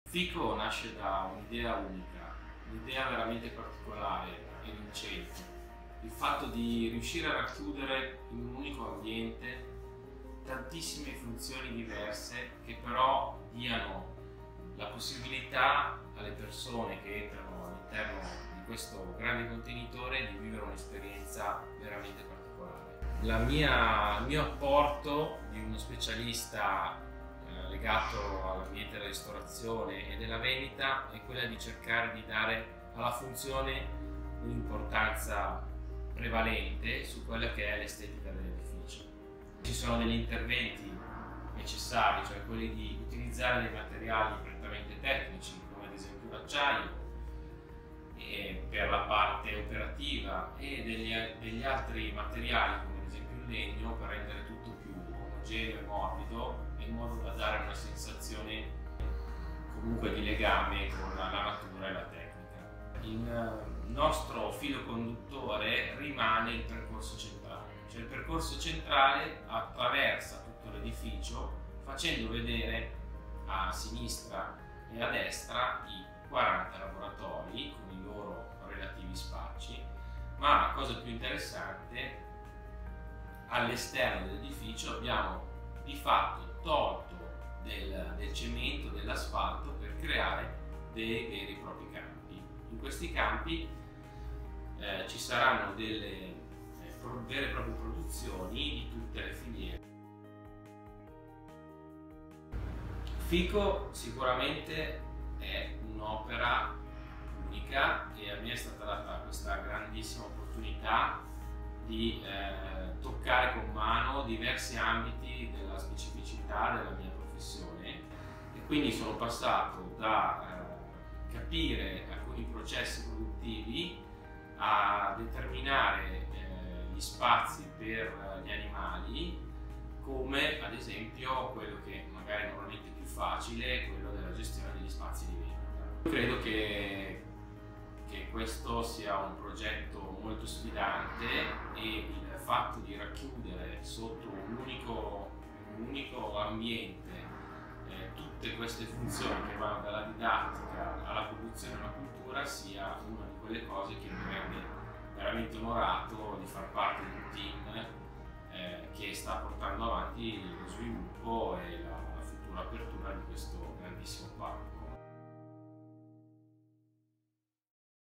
FICO Eataly World dagli occhi dei protagonisti - Thomas Bartoli
Il racconto di Thomas Bartoli, Architetto progettista di FICO Eataly World e Responsabile Eataly Design, sulle scelte progettuali e le fasi di realizzazione di Fabbrica Italiana Contadina.

"FICO è biodiverso ma unico. La sfida è stata proprio unire il paese paesaggisticamente e culturalmente più diversificato al mondo, in un unico luogo. Un luogo che fosse prima di tutto vero e che rispettasse insieme l’eterogeneità dei nostri soggetti e della nostra enogastronomia. La percorrenza quindi, il fil rouge narrativo, avviene nell’area centrale, mentre l’attrattiva e i passaggi di varie zone viene determinata e scandita per la maggior parte dal prodotto e da un codice colori che abbiamo istituito sia nei materiali sia nella comunicazione grafica. Questo meccanismo di percorso porta il visitatore da un mondo all’altro ma in un unicum coerente. Ugualmente, per quanto riguarda l’esterno saranno i vari campi a segnare i passaggi da un mondo all’altro, da una filiera all’altra. All’esterno parla la natura, parlano i campi, mentre all’interno parlano i prodotti".